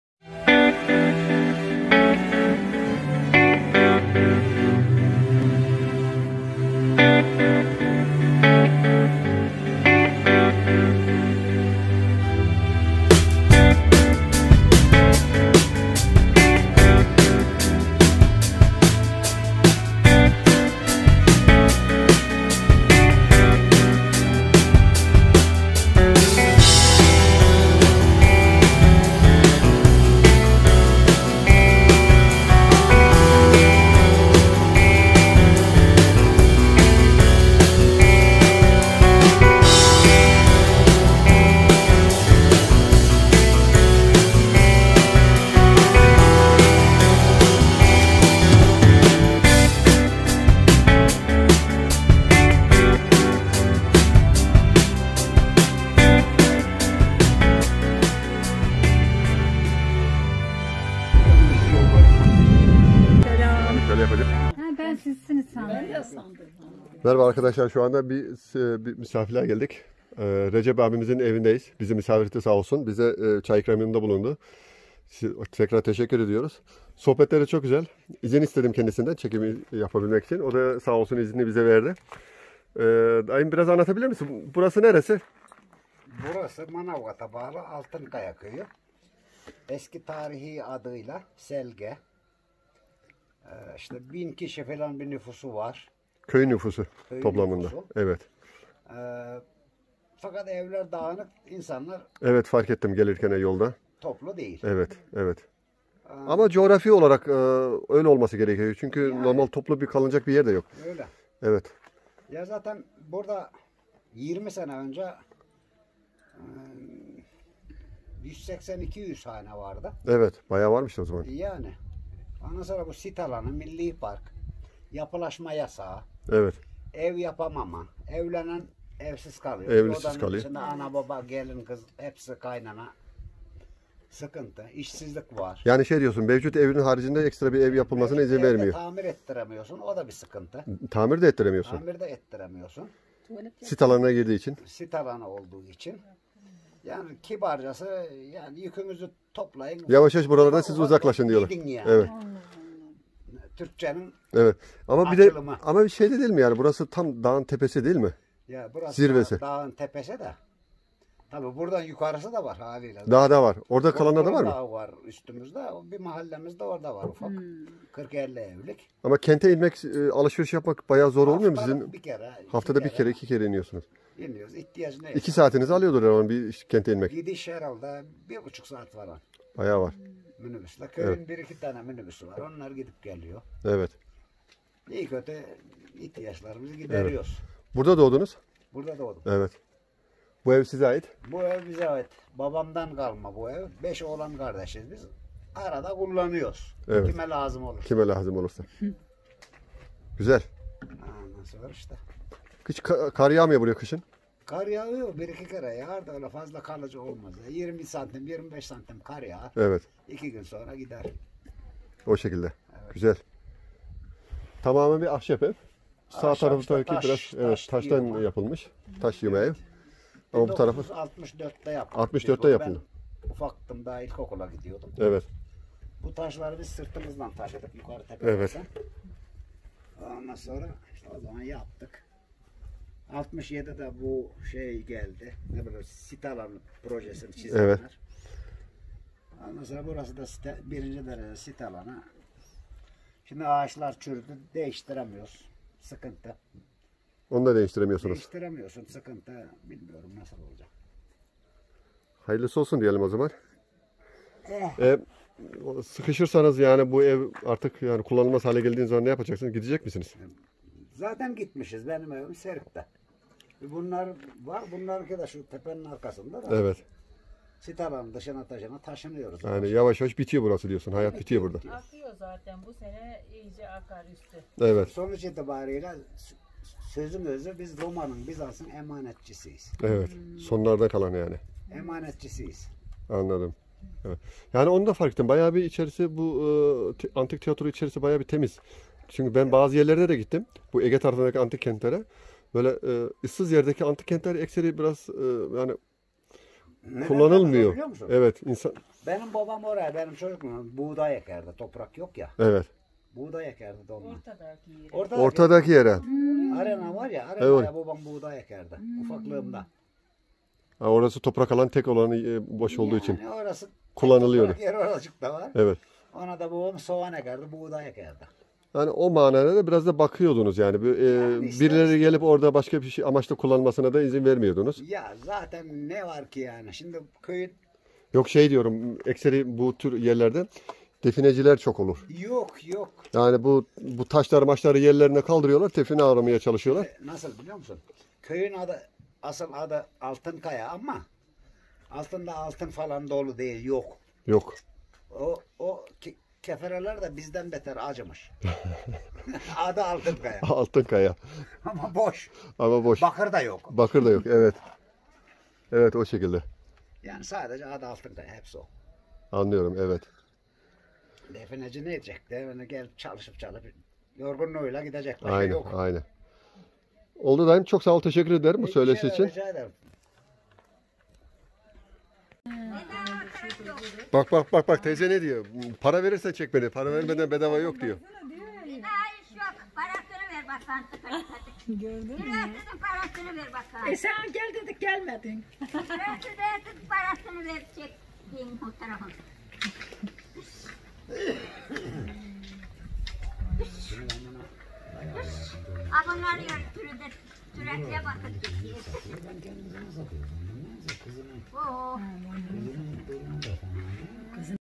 Mustafa Uc Photography / World trip with Musie antalya manavgat altınköyü selge)
Altın köyünde eski adı ile (Selgen) Recep Abimizin evinde kısa söyleşi.
SELGEN  tarihi bir yerleşim yeri ve 4 bin yılık bir mazisi var.